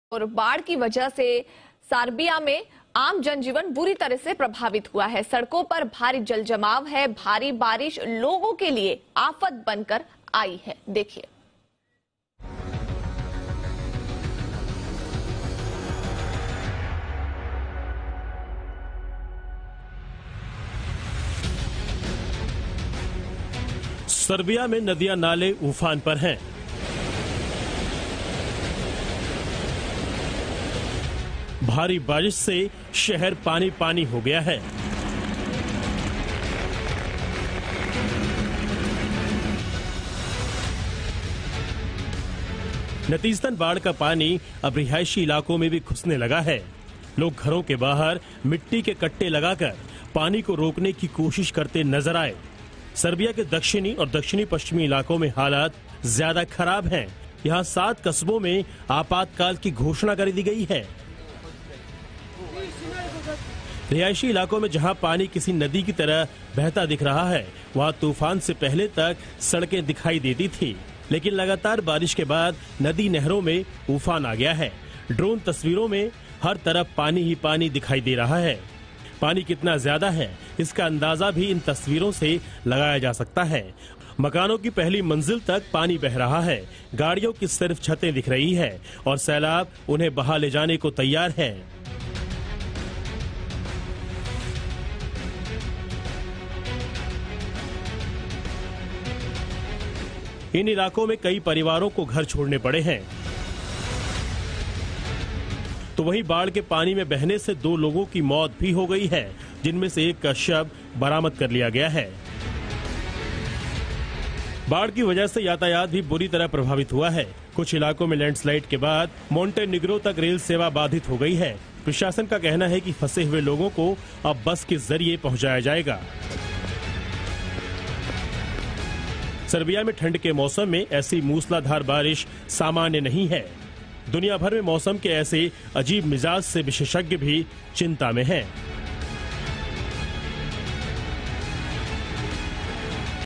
Heavy Rainfall & Floods in Serbia, Emergency has been declared in 7 towns after heavy water logging
Serbia में बाढ़ की वजह से आम जनजीवन बुरी तरह प्रभावित हुआ है, इस भारी जल जमाव के बाद 7 कस्बों में आपातकाल की घोषणा की गई है